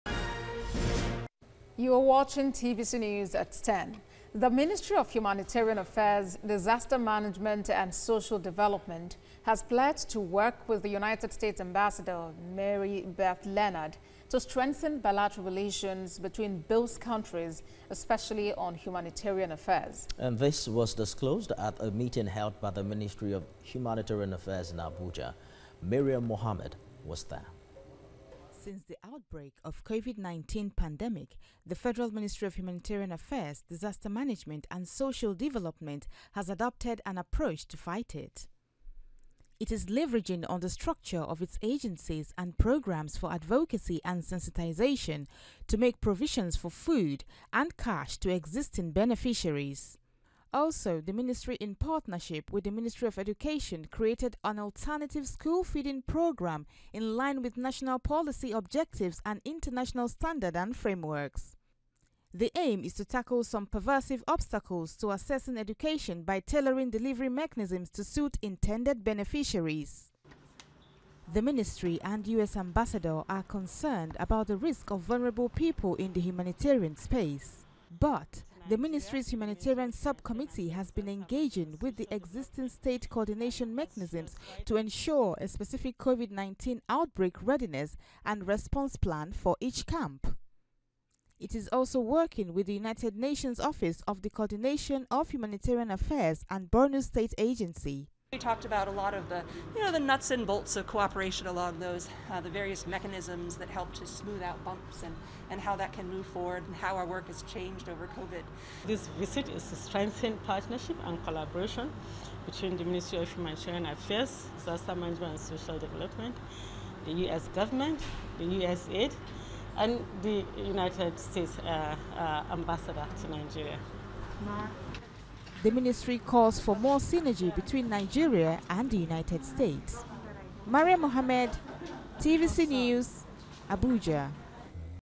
Nigerian Humanitarian Ministry, US Embassador Collaborate To Fight COVID-19
The Ministry of Humanitarian Affairs , Disaster Management and social
Development has pledged to work with the United States Ambassador Mary Beth Leonard, to strengthen bilateral relations relations between both countries especially on humanitarian affairs.

This was disclosed at a meeting held by the ministry of Humanitarian
Affairs in Abuja.